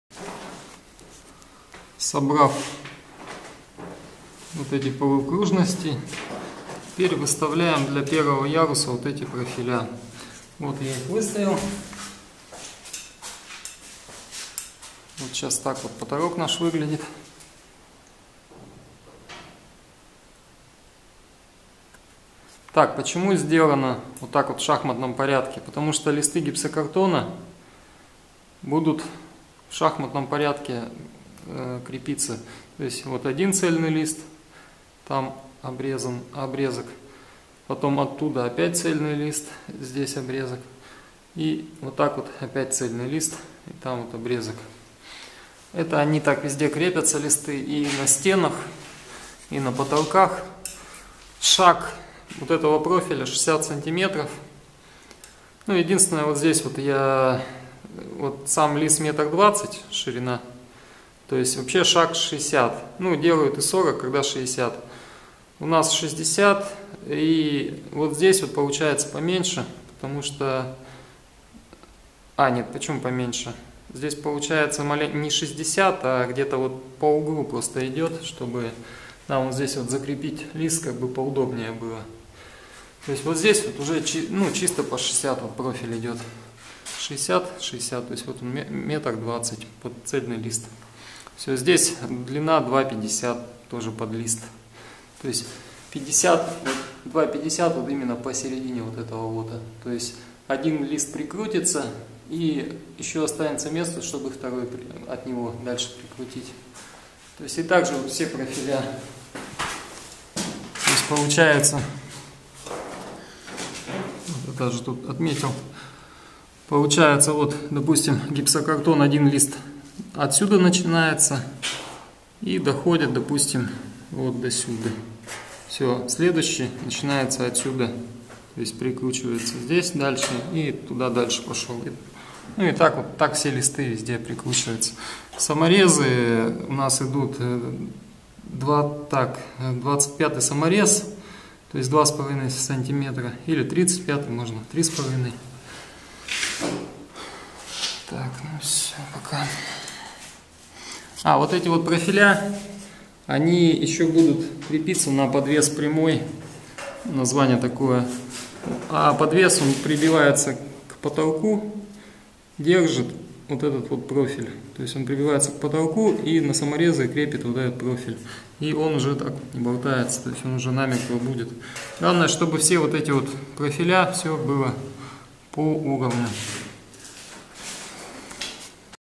Самоучитель по монтажу. Потолок из гипсокартона с подсветкой и светильниками -Урок 10
В данном видеоуроке вы узнаете :
1. Как собирается двухярусный потолок из гипсокартона.
2. Как прикручивается профиль на потолке при сборке листов гипсокартона в шахматном порядке.